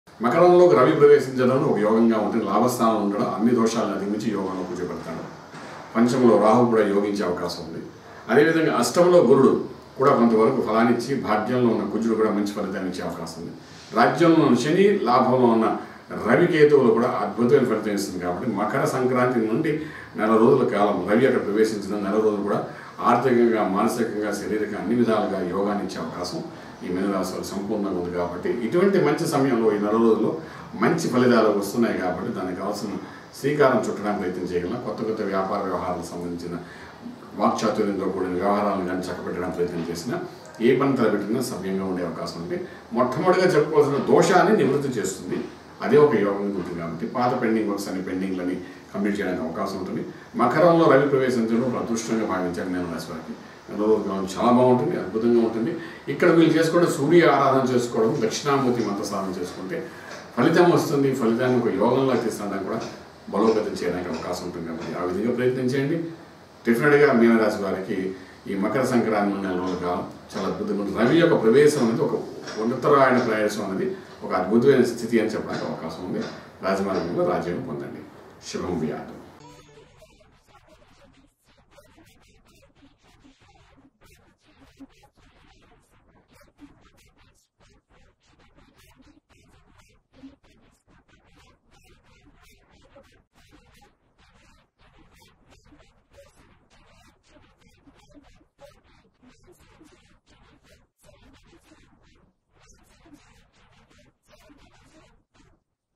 మీన రాశి వారికి జ‌న‌వ‌రి 15 త‌రువాత‌ క్రొత్త కార్యాలకు అనుకూల సమయం | Meena Rasi | Rasi phalalu
మీన రాశి వారికి జ‌న‌వ‌రి 15 త‌రువాత‌ క్రొత్త కార్యాలకు అనుకూల సమయం
Meena Rasi Rasi phalalu
Sri Mantha suryanarayana sharma
Astro Village